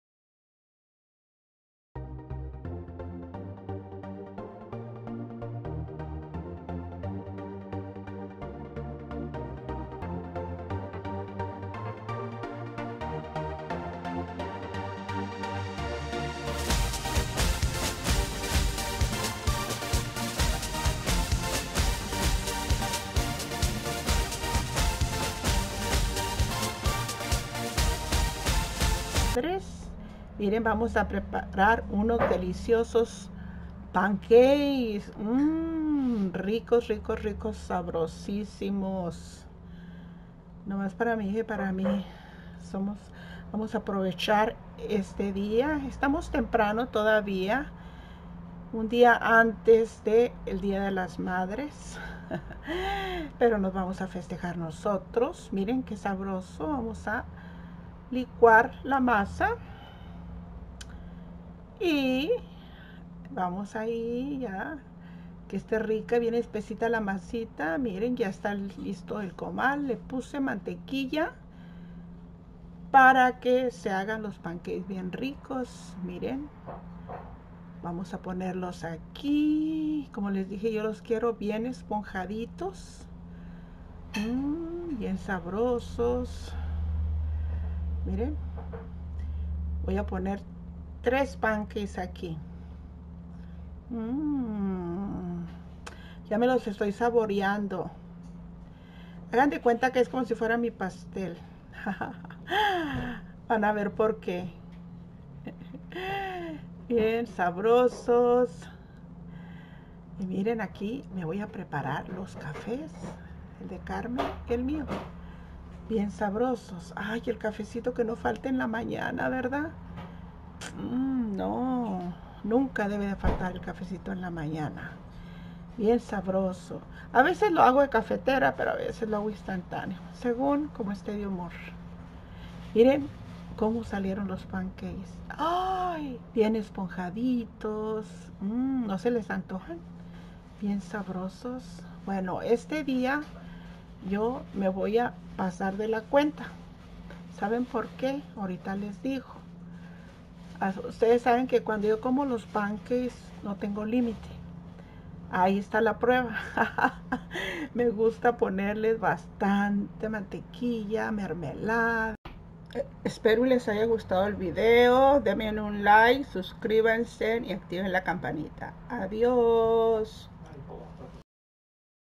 CONCINTIENDOME POR EL DIA DE LAS MADRES.
Hoy prepare un almuerzo especial para mi hija y para mi  por el dia de las madres.